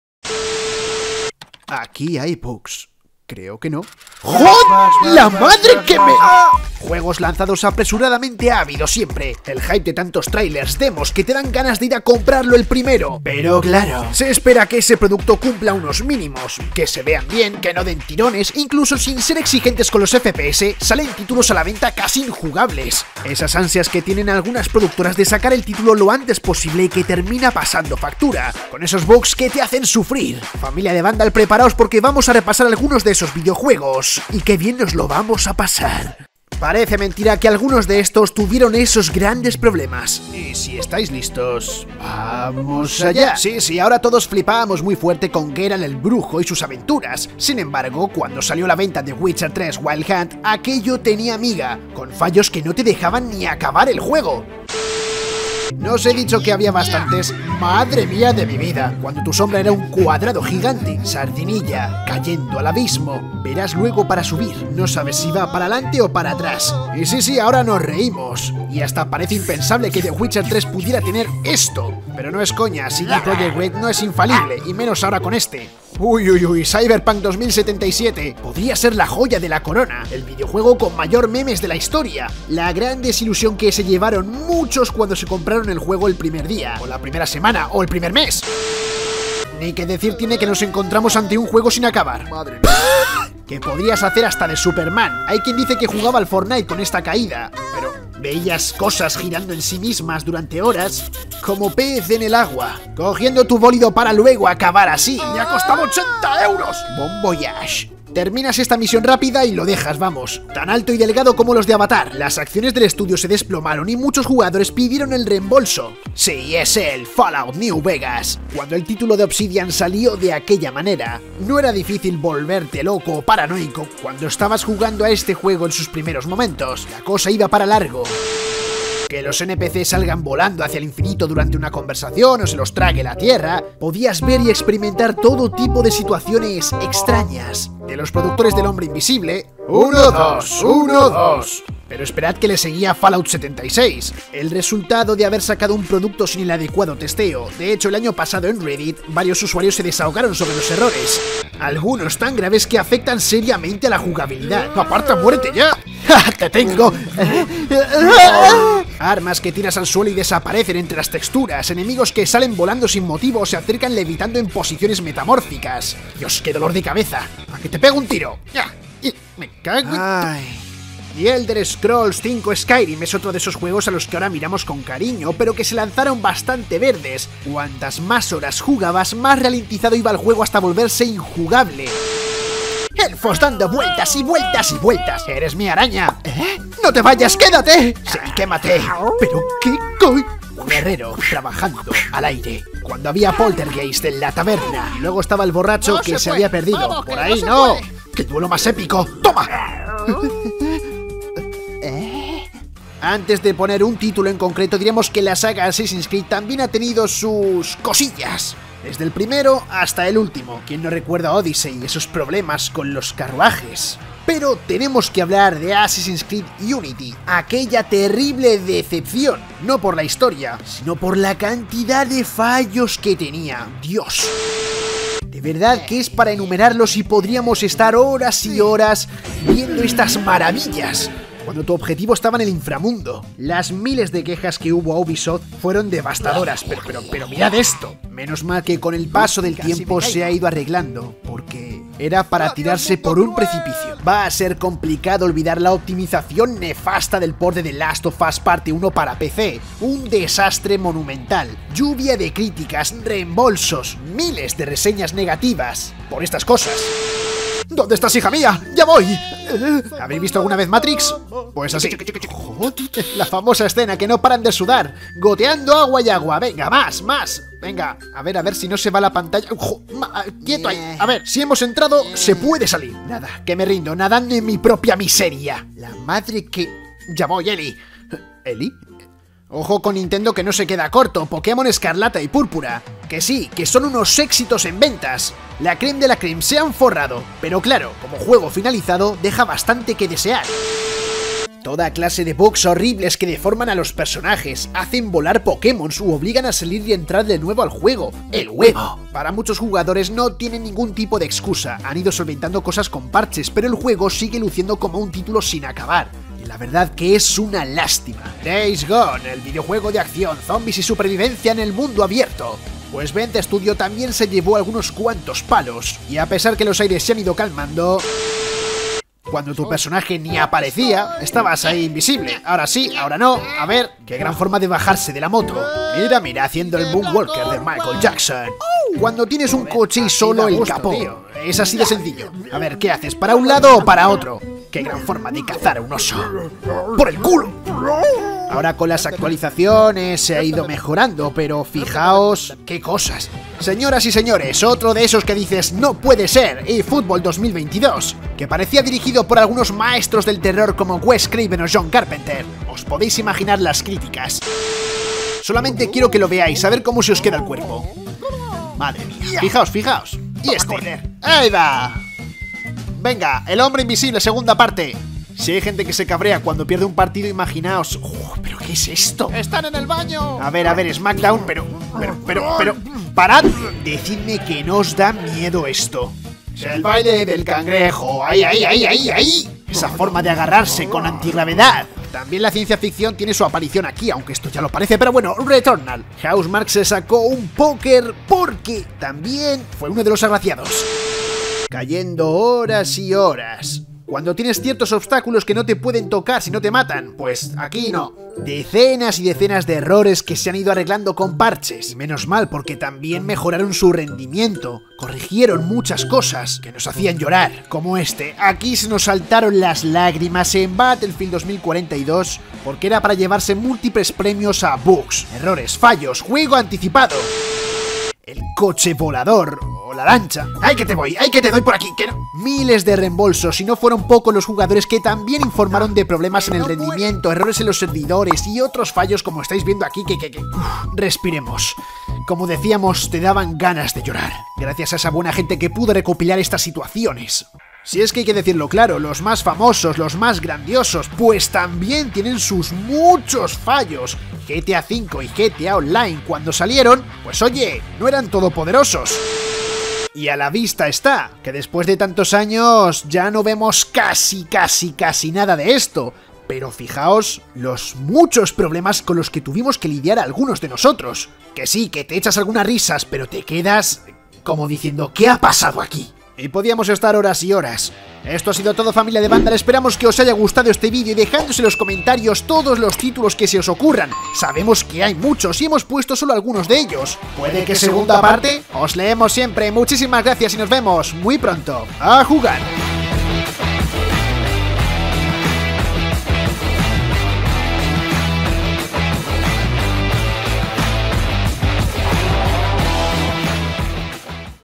VIDEOJUEGOS que salieron ROTOS: Un MAL DEMASIADO FRECUENTE
En lo que va de año ya hay varios títulos que han salido con problemas graves de rendimiento, pero esto no es un mal nuevo, juegos lanzados apresuradamente ha habido siempre; repasamos algunos.